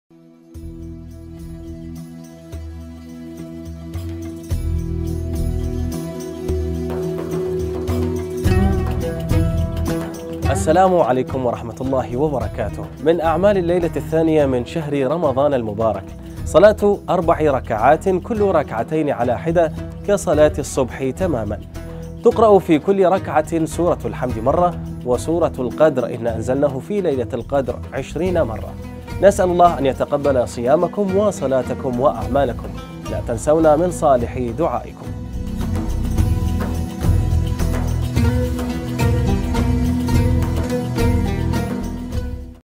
أعمال شهر رمضان المبارك صلاةُ اللّيلة الثّانية
أعمال شهر رمضان المبارك

صلاةُ اللّيلة الثّانية:
أربع ركعات، في كلّ ركعةٍ بعد الحمد يقرأ عشرين مرّة (إنّا اَنْزَلناهُ).

⏳الخميس 2 رمضان المبارك 1439هـ ـ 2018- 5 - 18 م